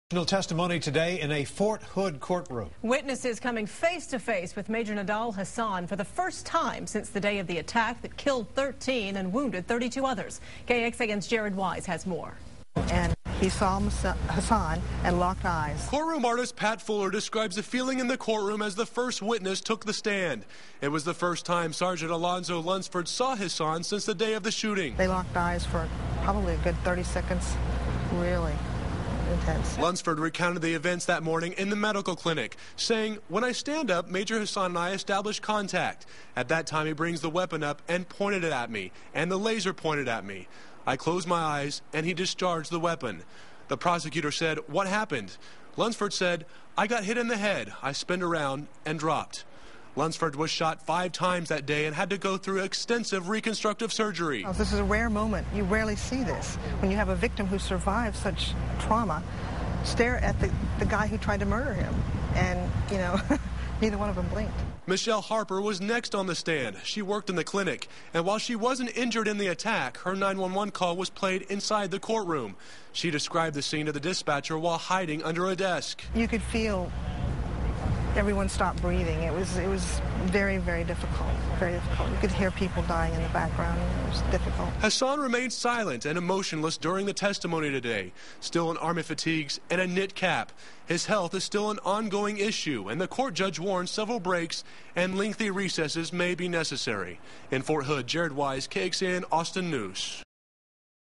Fort Hood shooting witness: 'The worst horror movie'
Witness in Hasan hearing: "He turned me around and looked me straight in the eyes and said, 'No, you've been shot.' I turned around and looked, and panic took over me.'"